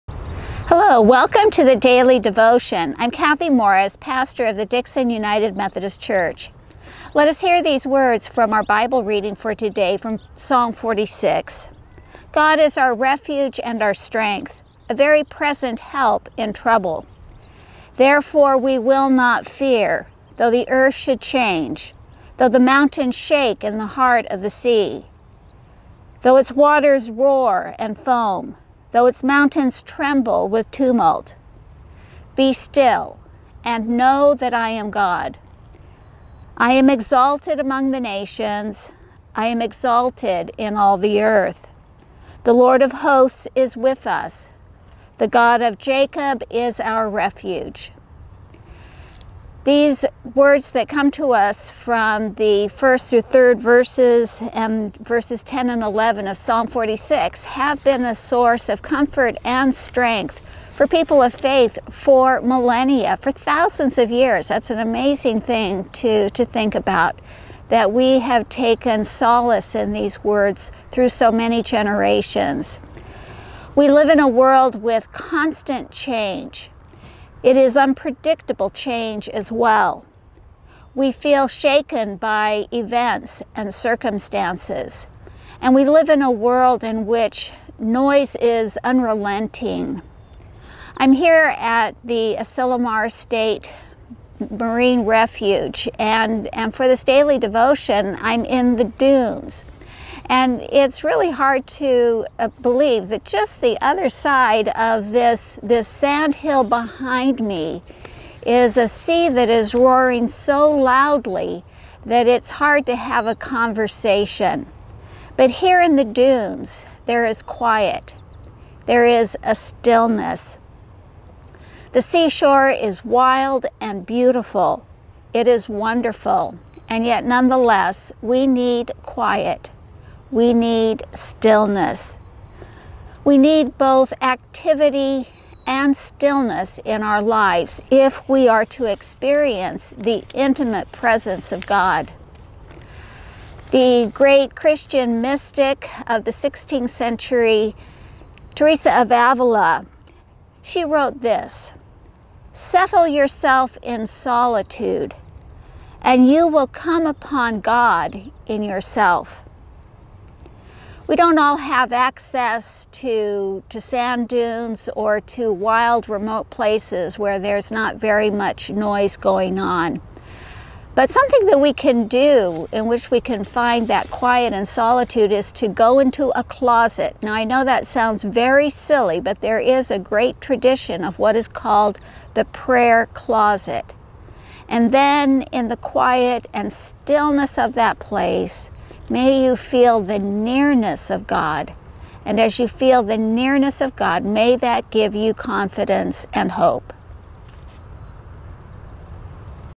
Daily Devotion May 17, 2021
Bible Reading: Psalm 46:1-3, 10-11

Teresa of Avila (16th century):
   Settle yourself in solitude and you will come upon God in yourself.

Take Action:
For your prayer time, find a very quiet place. You may want to try a closet. The quiet and closeness may give you a sense of God’s intimate presence. As you feel God’s nearness, may you find confidence and hope.

In your journal, list your prayers:
For others
For ourselves